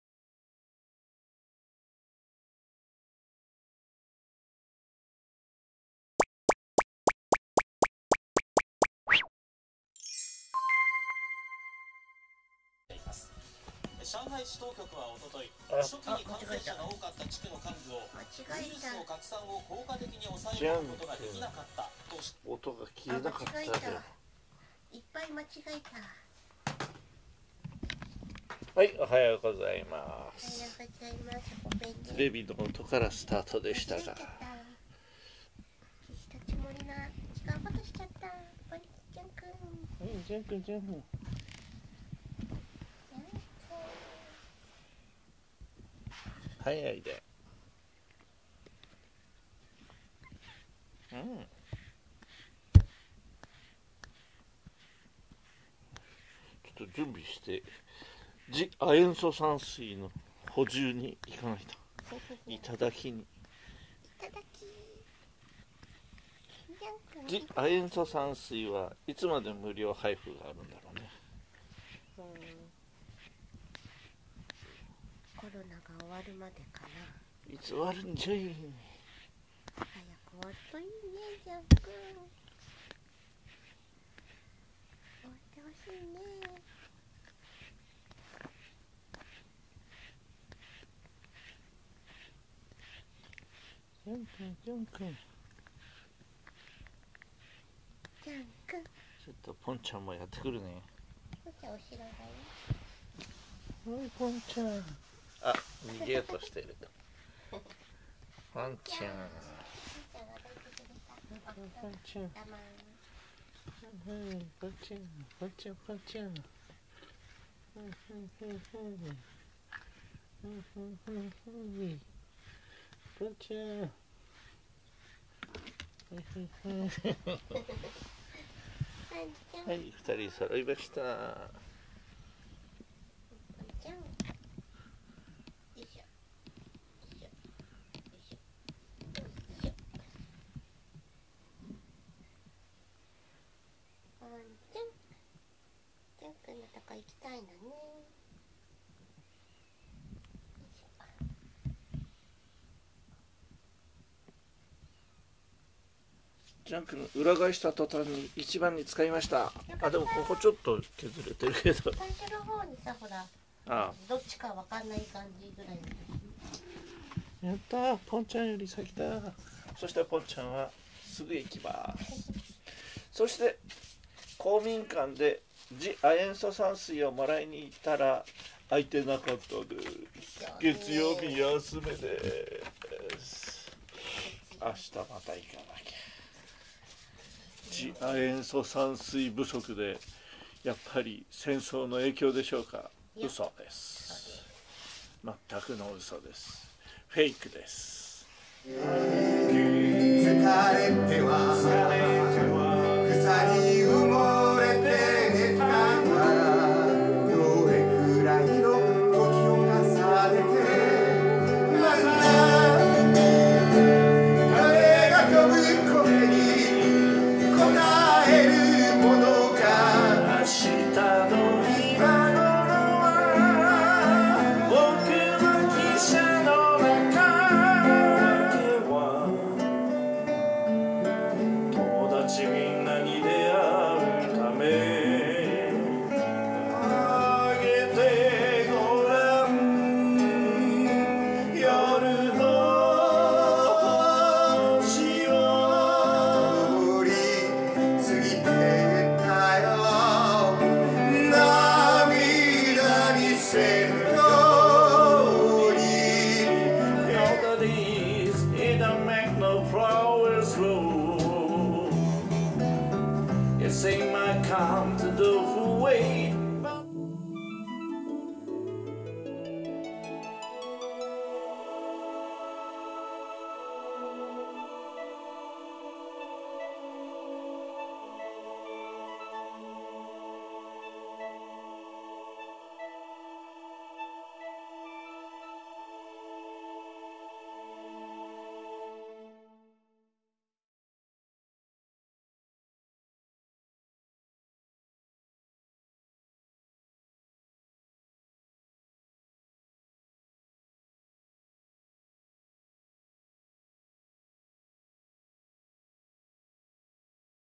ジャンくん次亜塩素酸水の無料配布は月曜休みだった【Jean & Pont 2815】2022/4/11 保護猫ジャンけんポン
もうかれこれ２年は除菌のための次亜塩素酸水を市の無料配布で賄っています。
朝８時半に公民館の入り口でタンクに入ったものをボトル１杯分だけいただいて帰るのですが、月曜日は公民館がおやすみでした。
コロナはまだ完全には収束していないので、ジャンくんやポンちゃんに持ち帰ることが内容に今でも使い続けています。
パパは歌のリハーサルに出かけました。